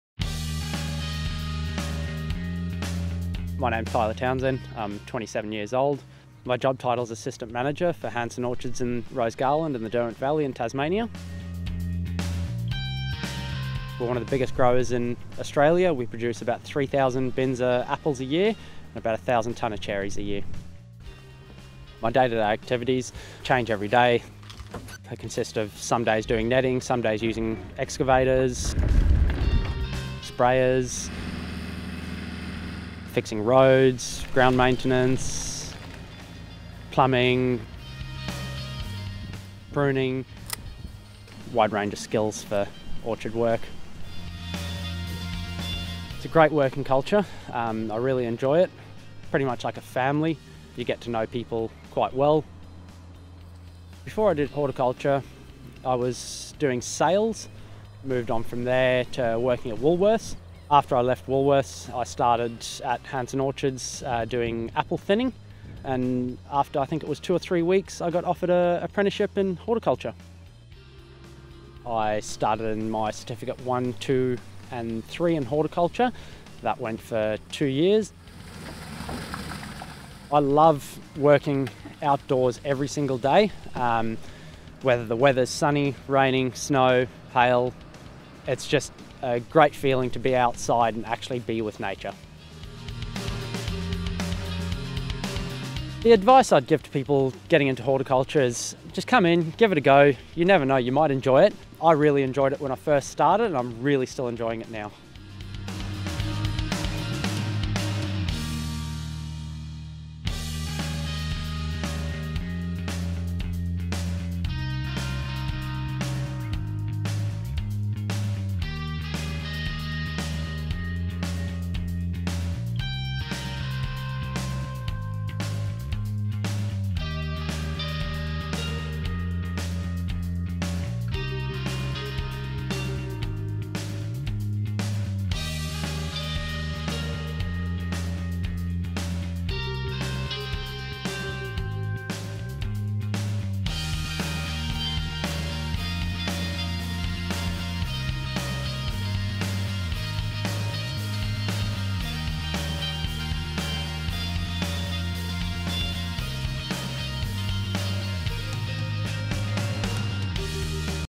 Horticulture – grow your career: Machinery Supervisor (Tyler Townsend, Hansen Orchards)
Tyler Townsend ~ HANSEN ORCHARDS, TAS

Hansen Orchards in southern Tasmania is a vertically integrated horticultural business growing, packing and marketing cherries and apples. As Assistant Manager / Machinery Supervisor, Tyler Townsend helps produce 3,000 bins of apples and approximately 1,000 tonnes of cherries each year. His job role is quite varied and sees him operate and maintain a wide-range of machinery including excavators, tractors, forklifts and sprayers. Tyler is responsible for netting, spraying, fixing roads, ground maintenance, and pruning in and around the orchard.

These videos show the daily tasks, responsibilities, and pathways of current employees on Australian farms